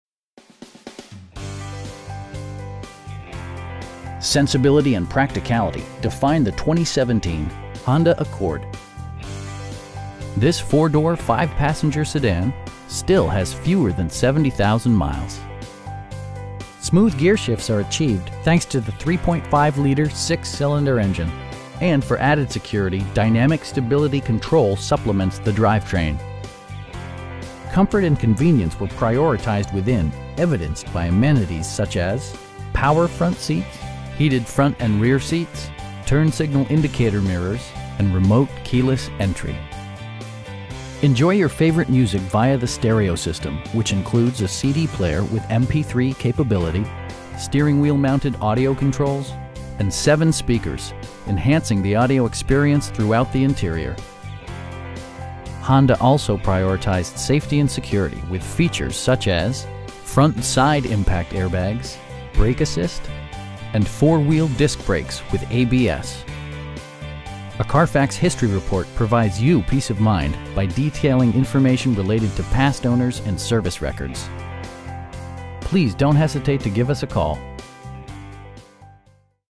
2017 Honda Accord Touring in Tampa, FL 33614
4600 N Dale Mabry Hwy

Sensibility and practicality define the 2017 Honda Accord. This 4 door, 5 passenger sedan still has fewer than 70,000 miles! Smooth gearshifts are achieved thanks to the 3.5 liter 6 cylinder engine, and for added security, dynamic Stability Control supplements the drivetrain. Comfort and convenience were prioritized within, evidenced by amenities such as: leather upholstery, power front seats, heated front and rear seats, power moon roof, turn signal indicator mirrors, lane departure warning, and remote keyless entry. Honda also prioritized safety and security with features such as: head curtain airbags, front side impact airbags, traction control, brake assist, ignition disabling, an emergency communication system, and 4 wheel disc brakes with ABS. Adaptive cruise control maintains a pre set distance behind the car ahead of you, simplifying highway driving and enhancing safety. A Carfax history report provides you peace of mind by detailing information related to past owners and service records. We know that you have high expectations, and we enjoy the challenge of meeting and exceeding them! Please don't hesitate to give us a call.